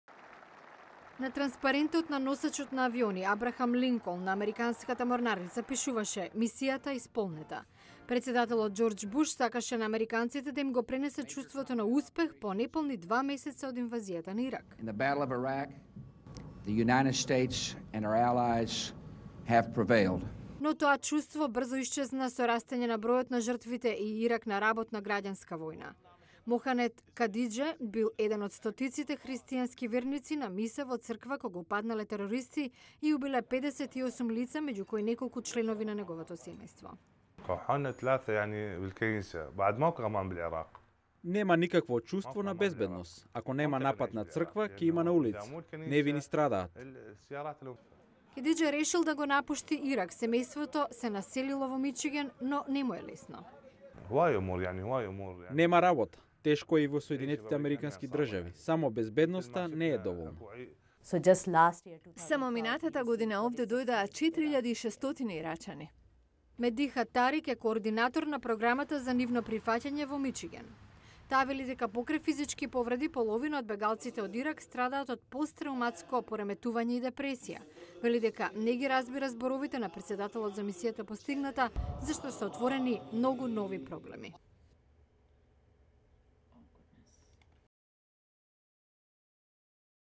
Годишнина на „Мисијата остварена"
Ситуацијата во Ирак десет години по познатата изјава на тогашниот претседател Џорџ В.Буш.